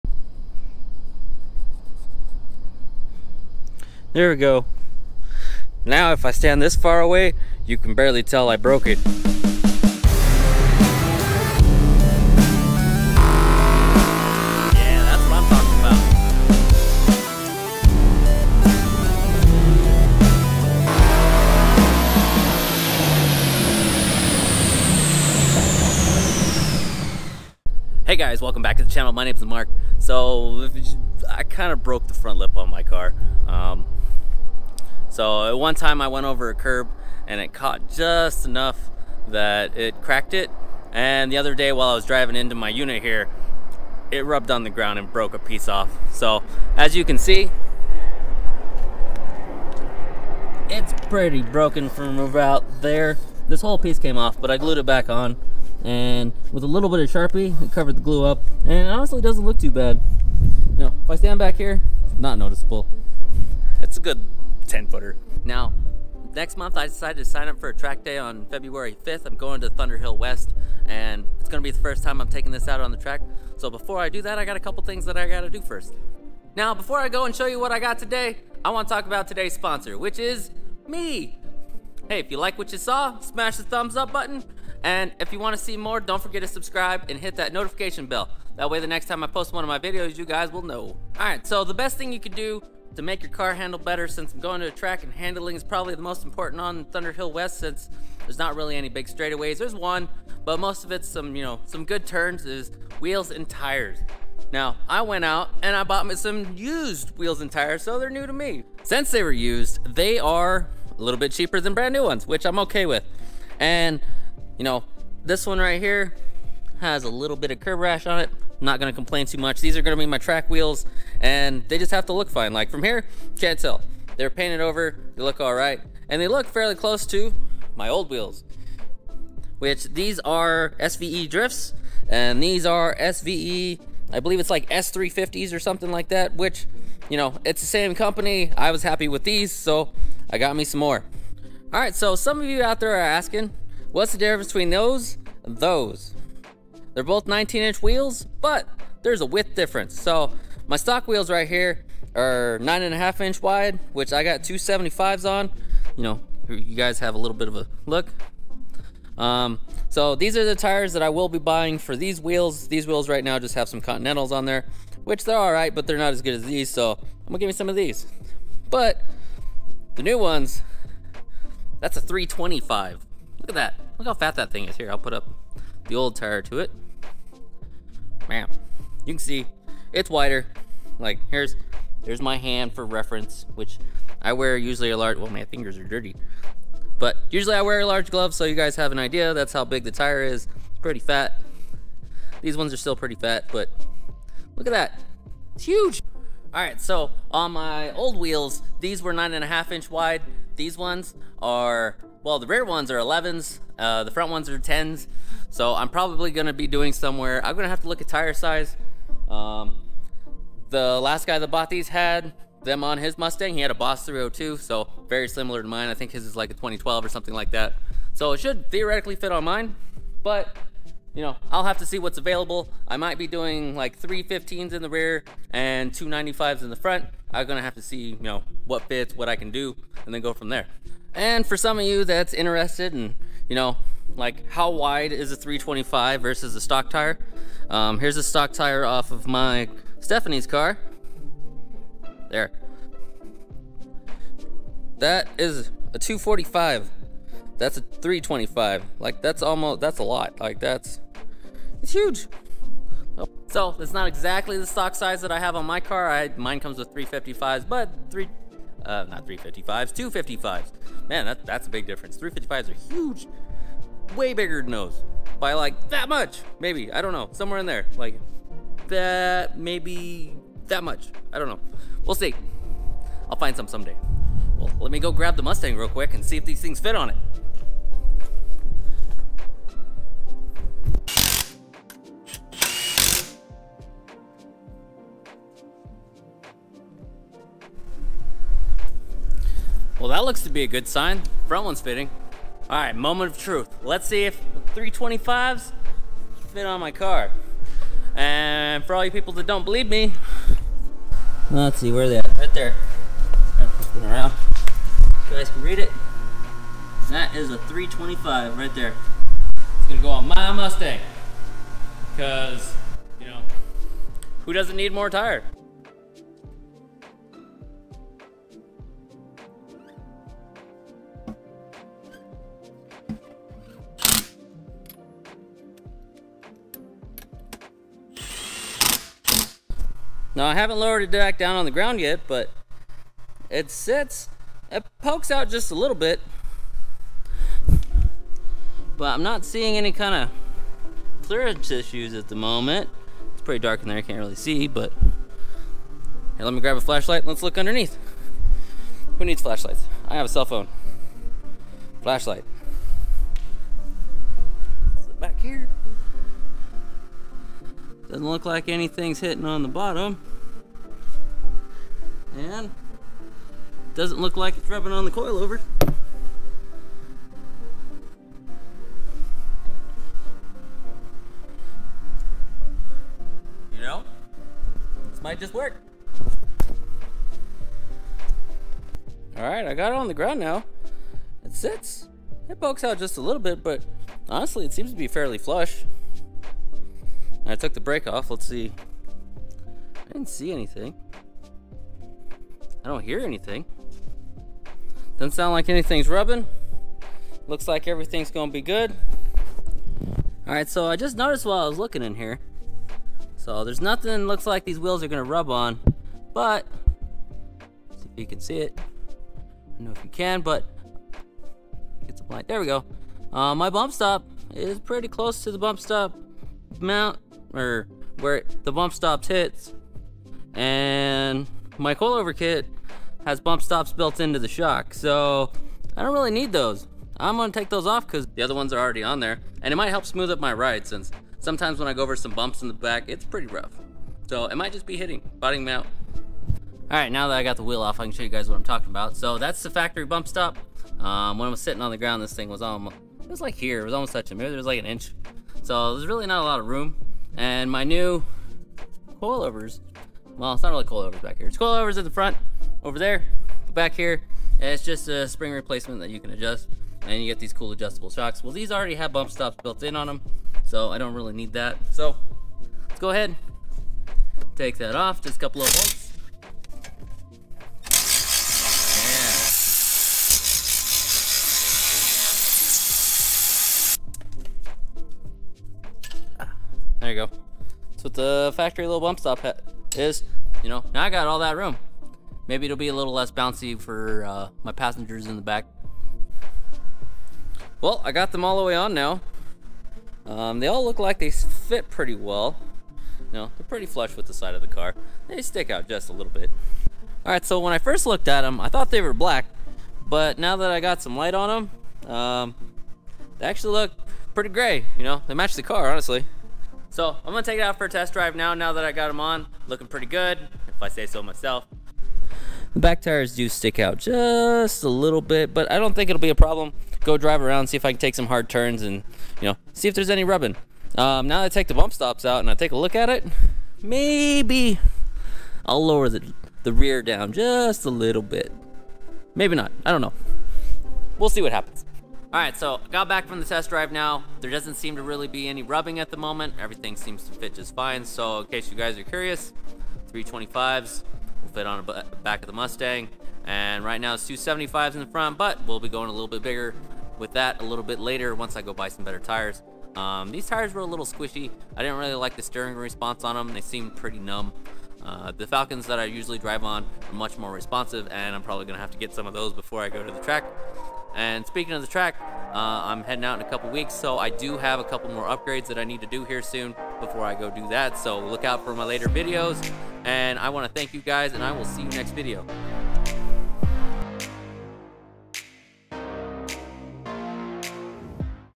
Installing Wider Wheels and Tires
I bought some bigger wheels for the mustang.  Trying them out to make sure they fit.